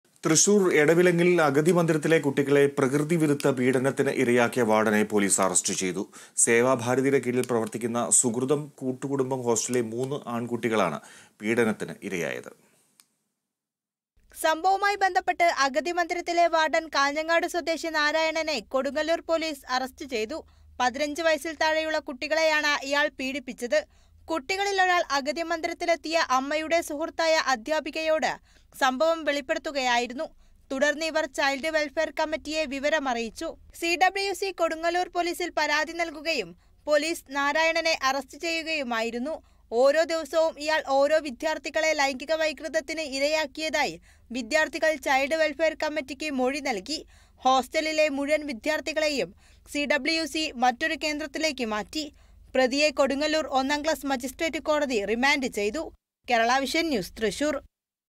അഗതിമന്ദിരത്തിലെ കുട്ടികളെ പ്രകൃതി വിരുദ്ധ പീഡനത്തിനിരയാക്കിയ വാര്‍ഡൻ അറസ്റ്റിൽ |THRISSUR
തൃശൂര്‍ എടവിലങ്ങില്‍ അഗതിമന്ദിരത്തിലെ കുട്ടികളെ പ്രകൃതി വിരുദ്ധ പീഡനത്തിനിരയാക്കിയ വാര്‍ഡനെ പൊലീസ് അറസ്റ്റ് ചെയ്തു. സേവാഭാരതിയുടെ കീഴില്‍ പ്രവര്‍ത്തിക്കുന്ന സുകൃതം കൂട്ടുകുടുംബം ഹോസ്റ്റലിലെ മൂന്ന് ആണ്‍ കുട്ടികളാണ് പീഡനത്തിനിരകളായത്.

About COA